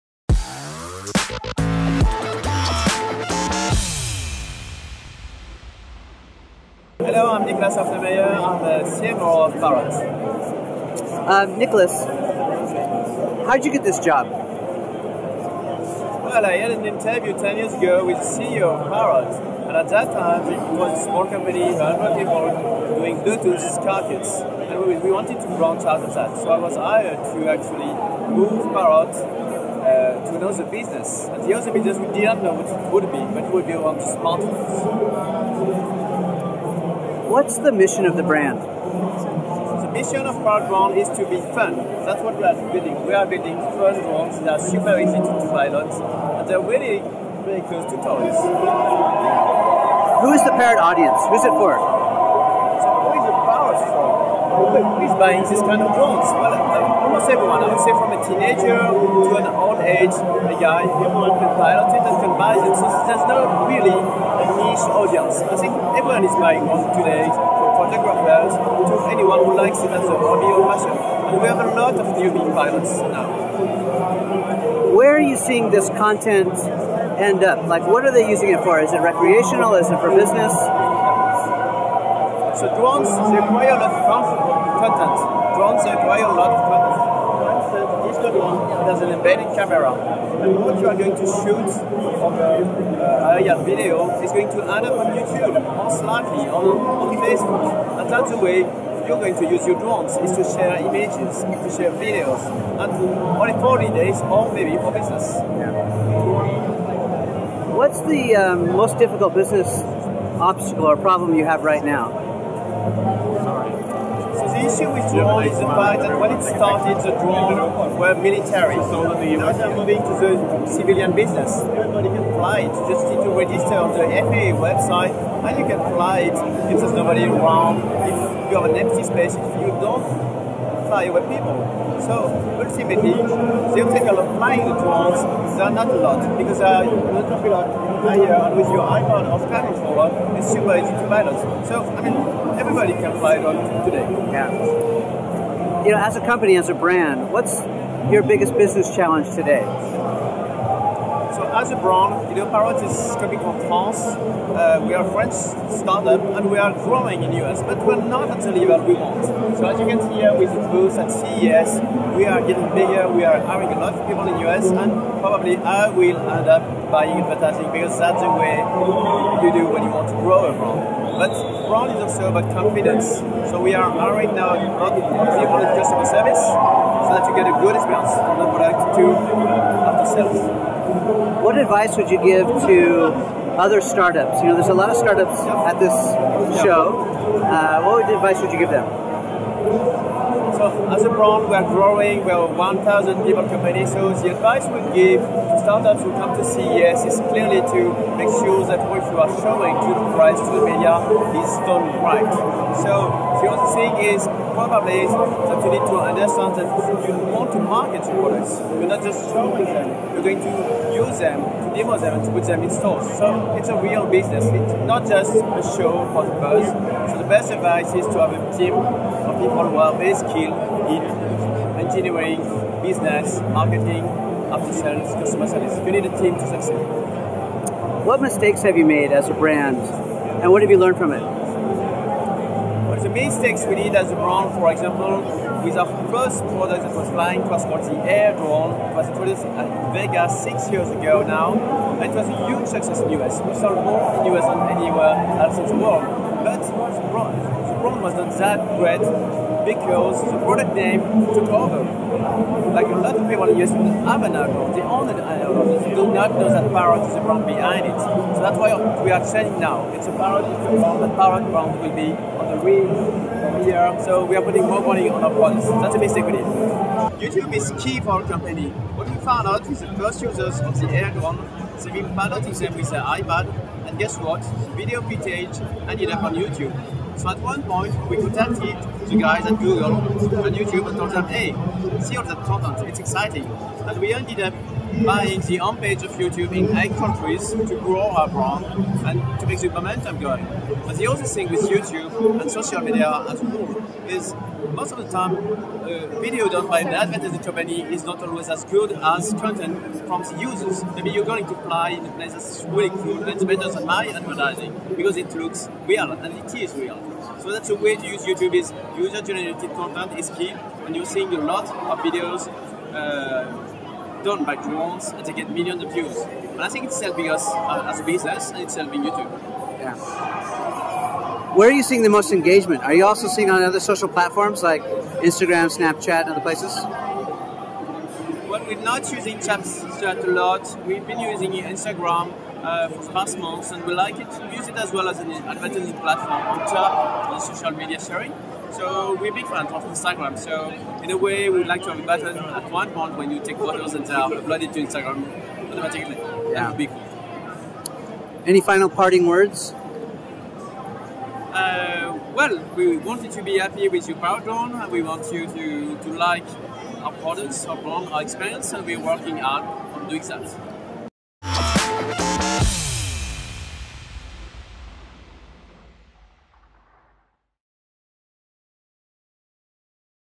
CES 2016 Panasonic 4K LUMIX Creators Studio - Nicolas Halftermeyer of Parrot
CMO Nicholas Halftermeyer of Parrot discusses the growth of drone technology and their use of video on YouTube and social media to reach customers.